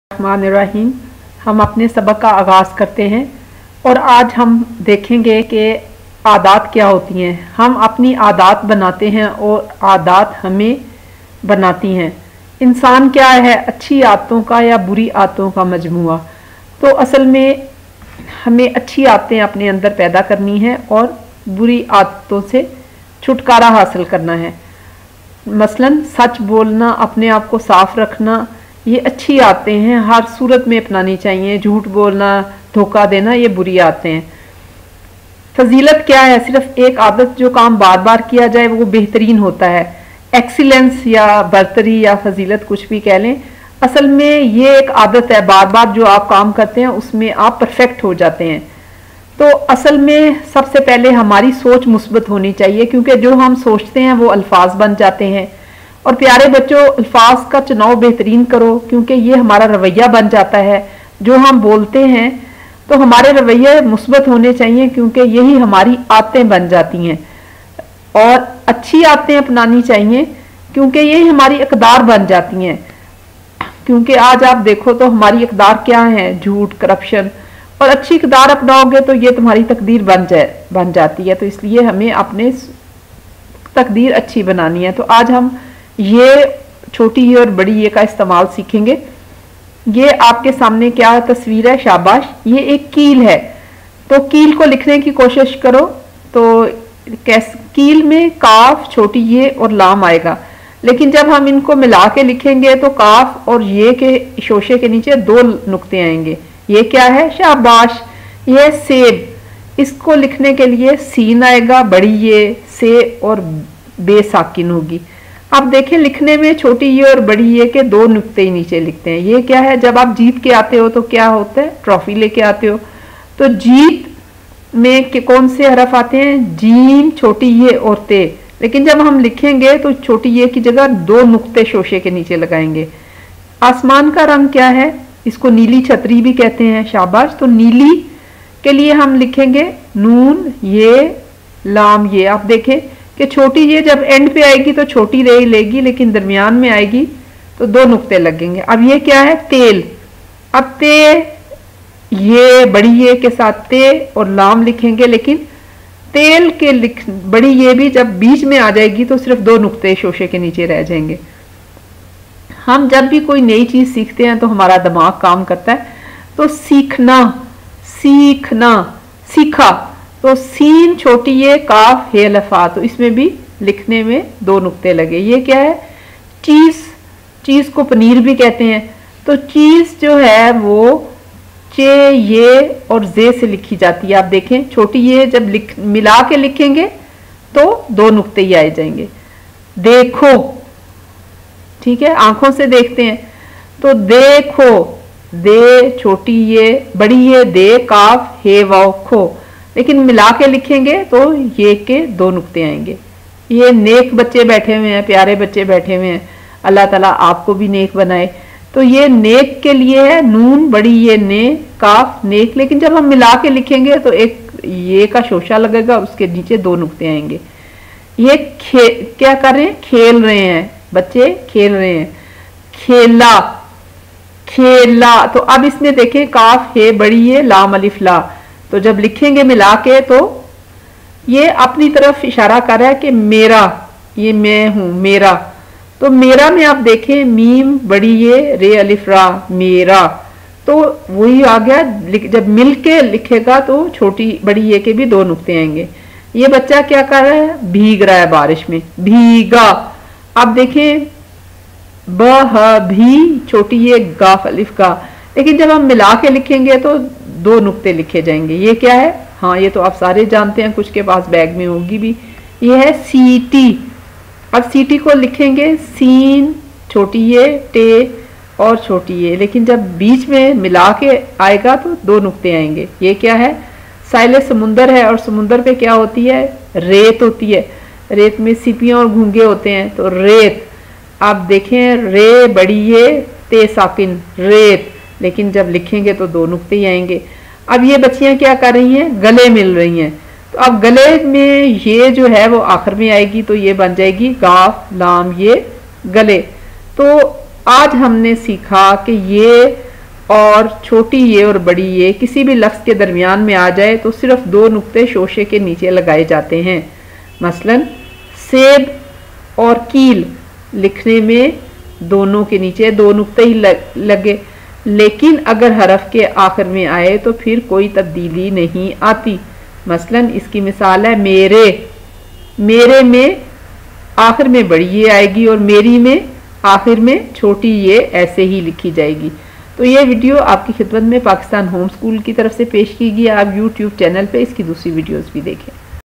Urdu Grammar/اردو سیکھیں ی اور ے کا استعمال
Pakistan home school presents Urdu haroof e Tahaji for beginners, Urdu Parhna seekhein
learn basic Urdu.
In this video you learn basic Urdu. Because Urdu is our national language, but mother tongue is different. Content is explained in simple way with meaning of difficult words according to need of beginners. Moral lessons in the content are also explained. no need for tuition.